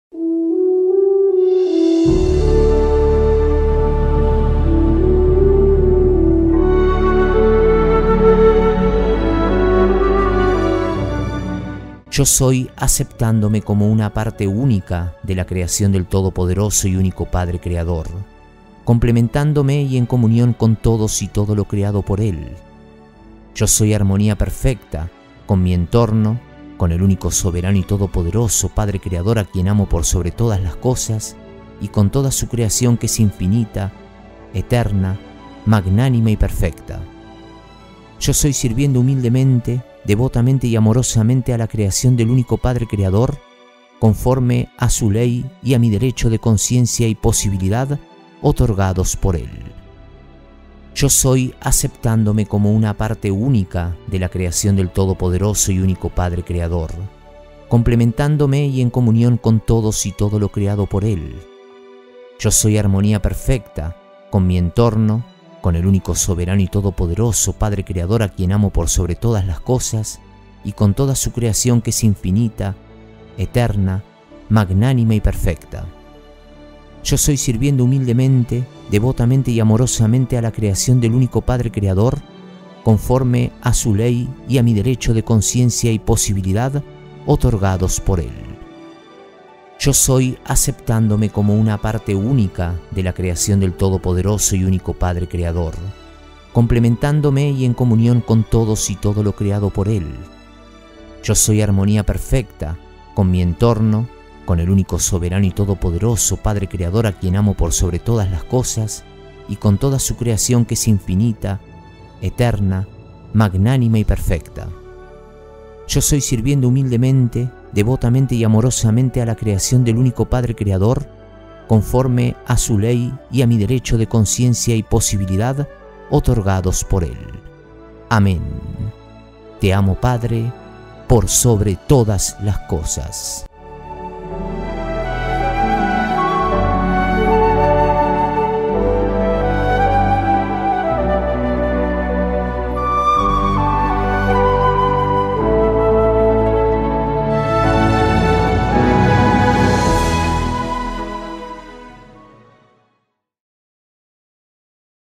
DECRETO DE ACEPTACION DE LA PERFECCION QUE YO SOY GRACIAS AL PADRE CREADOR
DECRETO DE ACEPTACION DE LA PERFECCION QUE
YO SOY GRACIAS AL PADRE CREADOR

YO SOY ACEPTANDOME COMO UNA PARTE UNICA DE LA CREACION DEL TODOPODEROSO Y UNICO PADRE CREADOR, COMPLEMENTANDOME Y EN COMUNION CON TODOS Y TODO LO CREADO POR EL. YO SOY ARMONIA PERFECTA CON MI ENTORNO, CON EL UNICO SOBERANO Y TODOPODEROSO PADRE CREADOR A QUIEN AMO POR SOBRE TODAS LAS COSAS Y CON TODA SU CREACION QUE ES INFINITA, ETERNA, MAGNANIMA Y PERFECTA.

YO SOY SIRVIENDO HUMILDEMENTE, DEVOTAMENTE Y AMOROSAMENTE A LA CREACION DEL UNICO PADRE CREADOR, CONFORME A SU LEY Y A MI DERECHO DE CONCIENCIA Y POSIBILIDAD OTORGADOS POR EL.

AMEN.

TE AMO PADRE POR SOBRE TODAS LAS COSAS.

ERIC